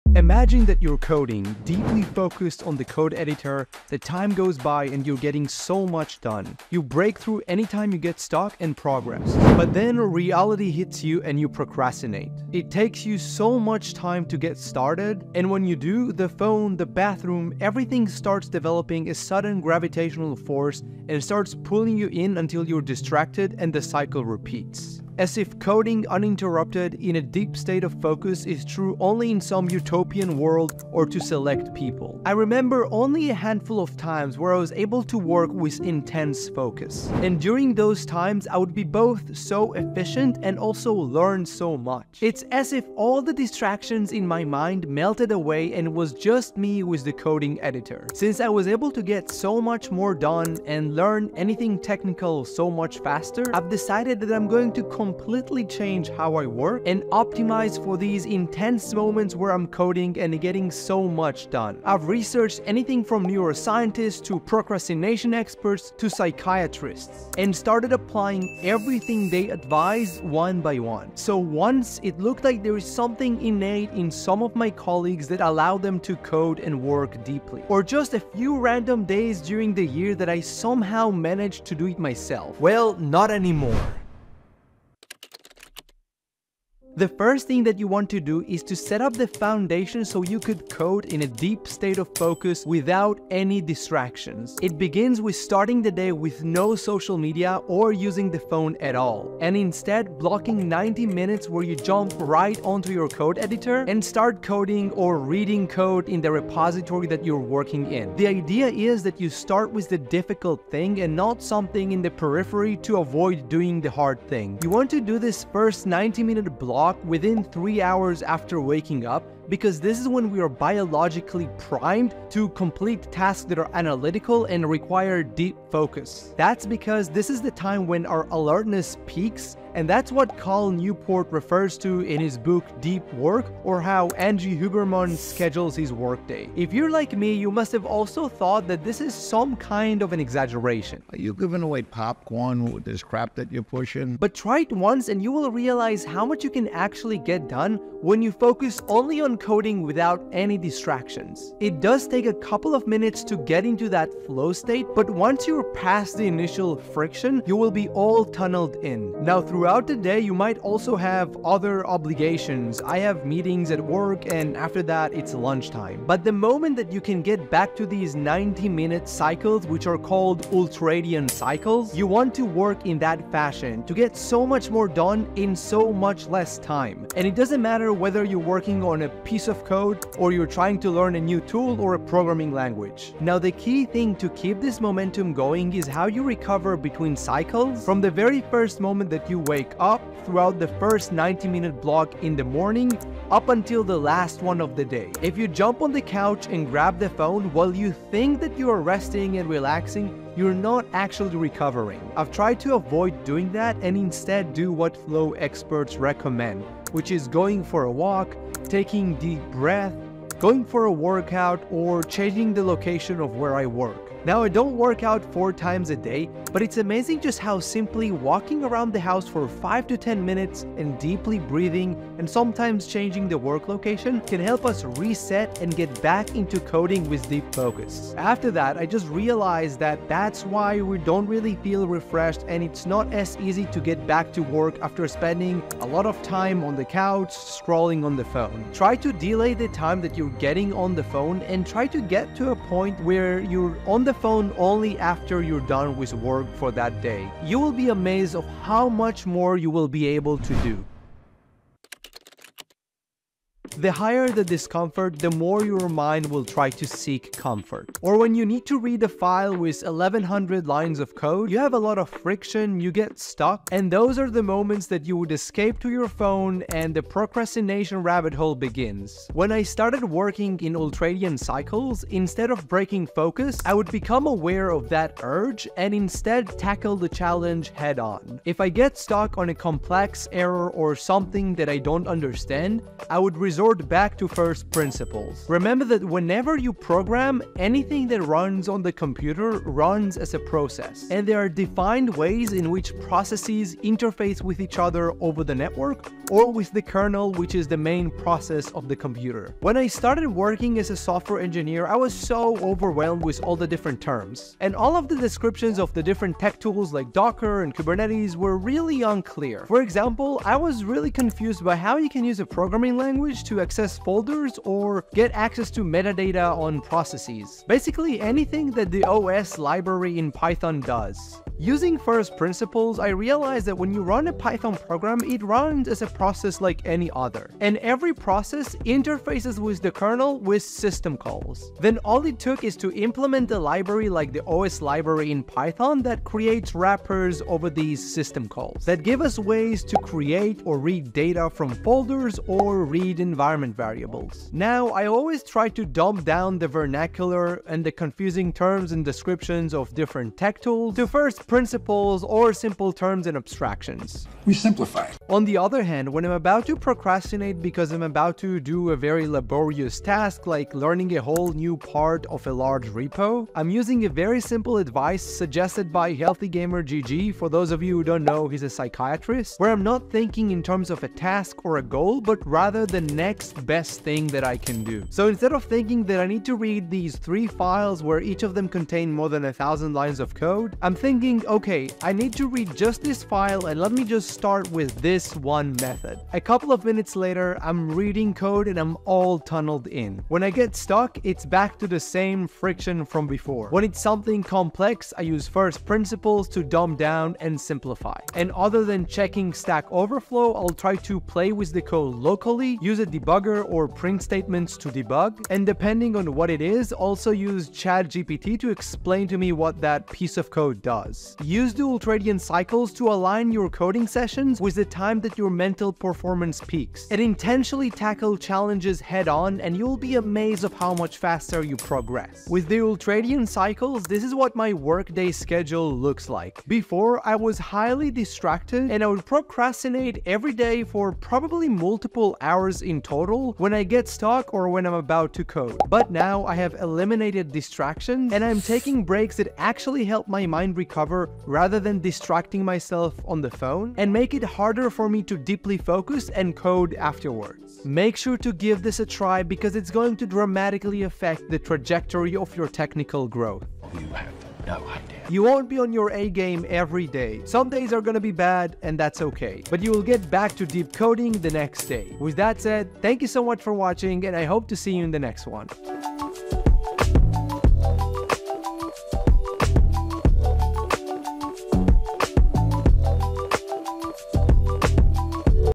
How To Learn Coding FAST & Stop Struggling
If there is ever a moment that feels like one of those programming scenes in the movies is when you're able to get into the "flow" state while coding. Since coding is something an engineer does most of the day for most days of the week, I've decided to put effort into adjusting my day-to-day, regardless of whether I work from home or from office, so that I could get into a flow of coding. Improving my performance aside, doing so showed me how much time I've wasted on context switching from checking the phone, or being distracted in any other way.